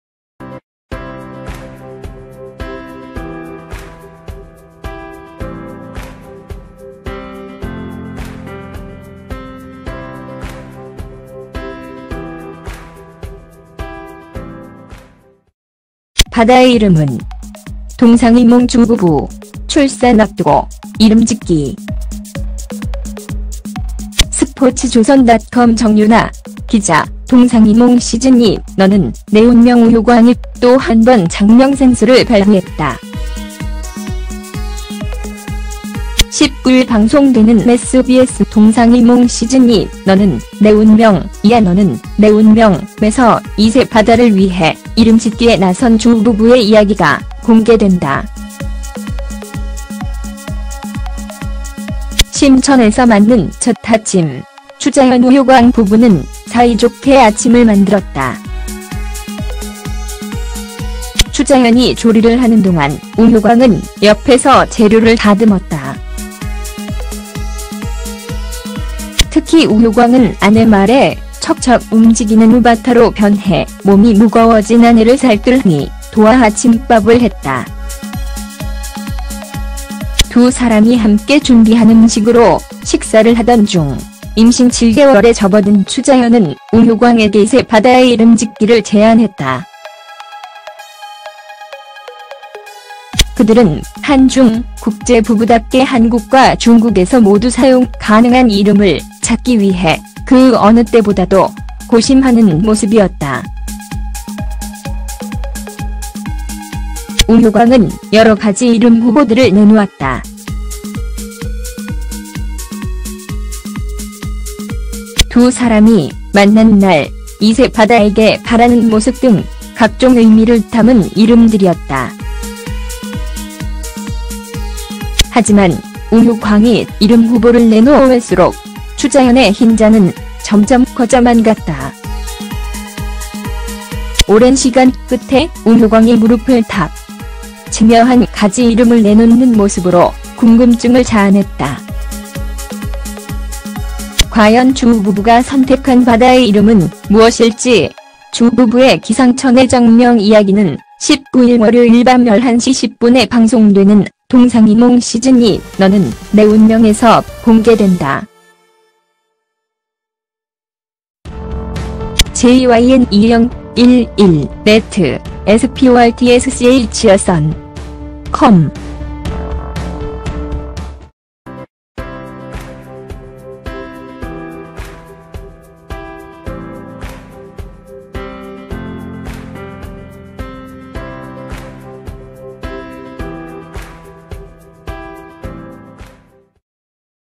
"'바다'의 이름은?"..'동상이몽' 추우부부, 출산 앞두고 이름짓기 - koreanbiz
[스포츠조선닷컴 정유나 기자] '동상이몽 시즌2 - 너는 내 운명' 우효광이 또 한번 '작명 센스'를 발휘했다. 19일 방송되는 SBS '동상이몽 시즌2 - 너는 내 운명'(이하 '너는 내 운명')에서 2세 '바다'를 위해 이름 짓기에 나선 추우부부의 이야기가 공개된다. 심천에서 맞는 첫 아침, 추자현-우효광 부부는 사이좋게 아침을 만들 - koreanbiz